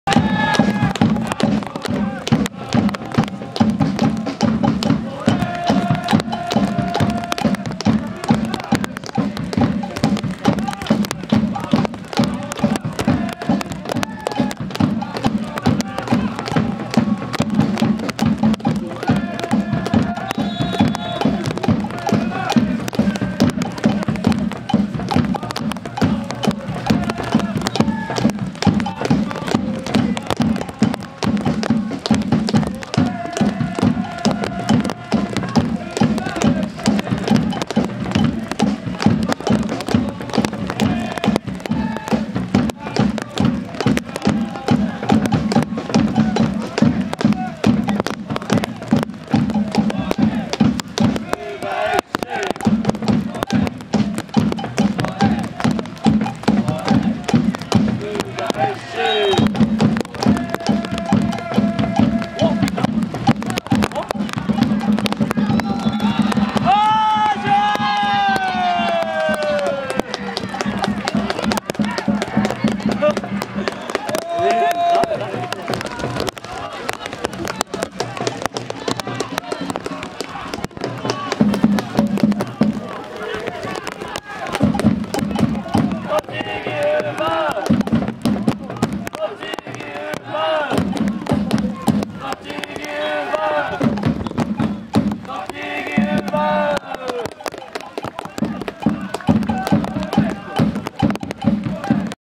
2012.9.23 JFL 第26節 栃木ウーヴァFC×FC琉球 2/3
2012年9月23日（日）　13：00Kickoff
JFL 第26節　栃木ウーヴァFC 1 － 1 FC琉球
栃木市総合運動公園陸上競技場

前半2分、岩切が直接FKを決め栃木ウーヴァが先制！